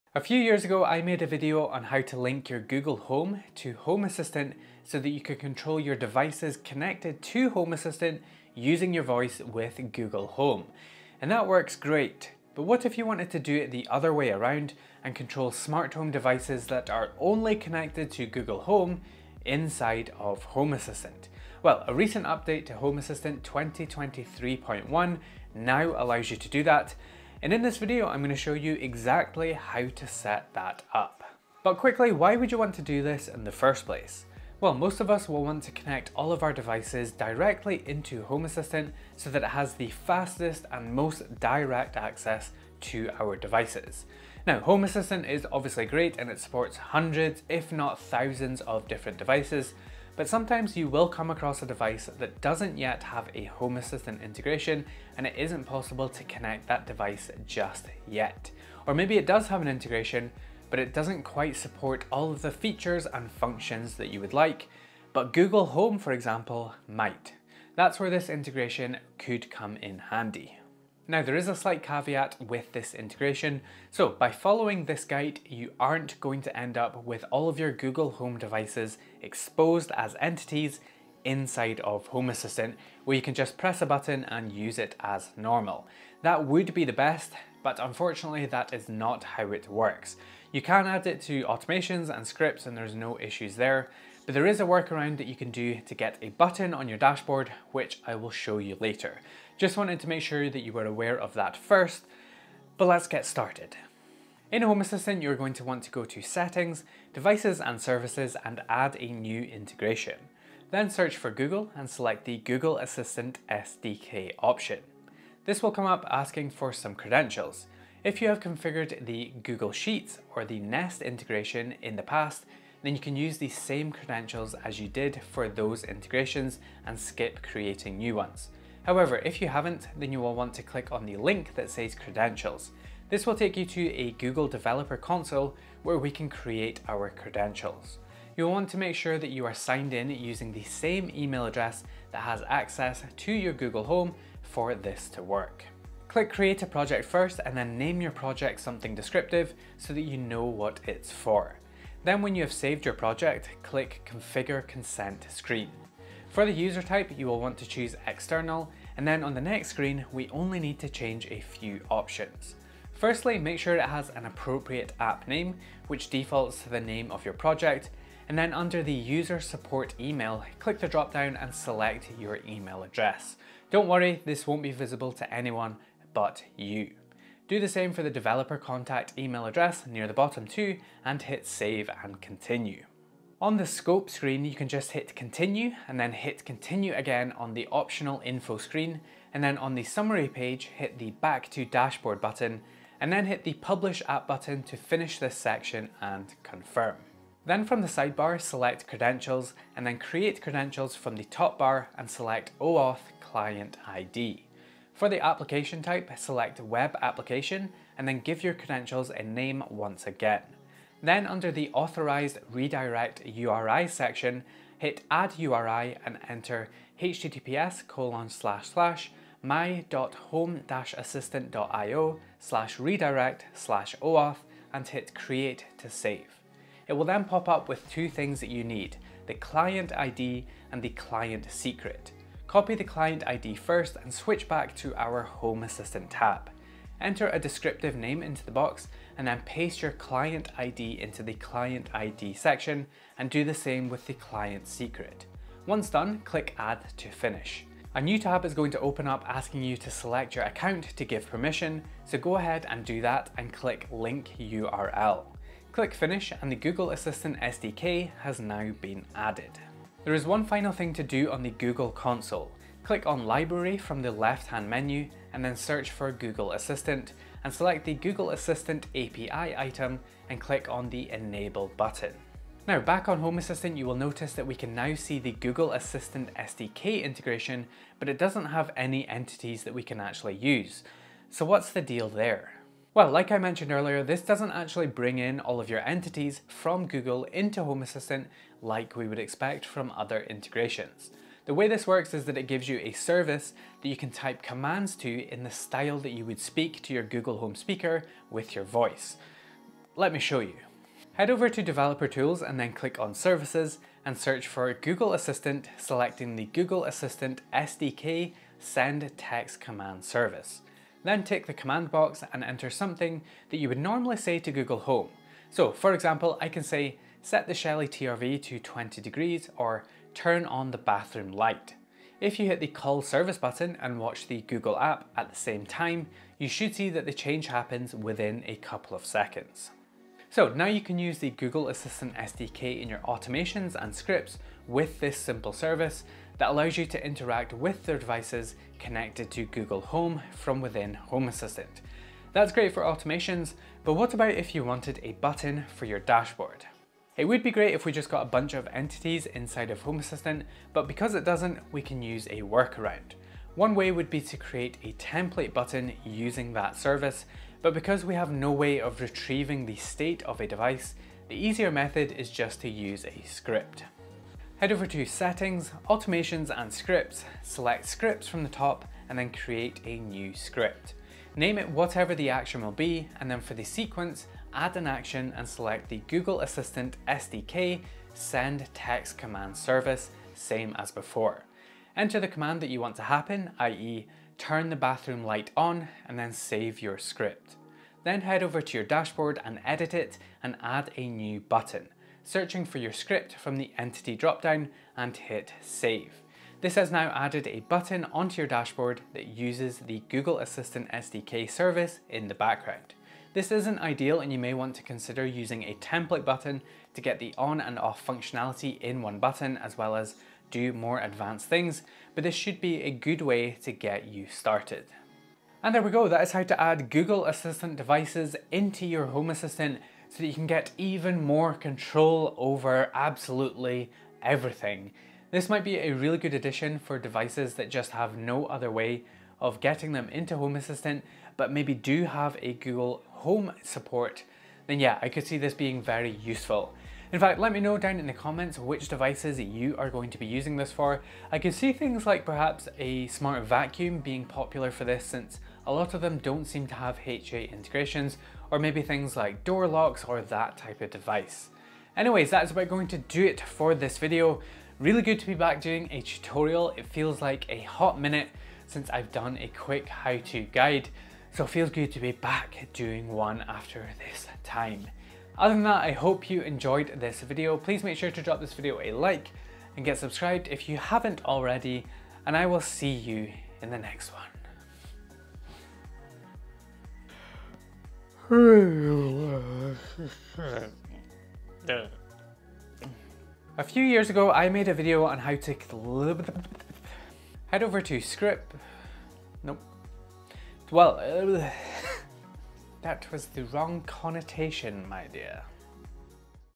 Controlling Google Home With Home Assistant!
A few years ago, I did a tutorial on how to link your Google Assistant to Home Assistant so that you can control devices connected to HA using Google Home. However, what if you wanted to do it the other way round and control your smart home devices that are only connected to your Google Home inside of Home Assistant? Thanks to a recent addition to Home Assistant 2023.1, you are now able to do just that & in this video, I show you how to set it up.

EVERYWHERE YOU CAN FIND ME:
Everything Smart Home Community Discord: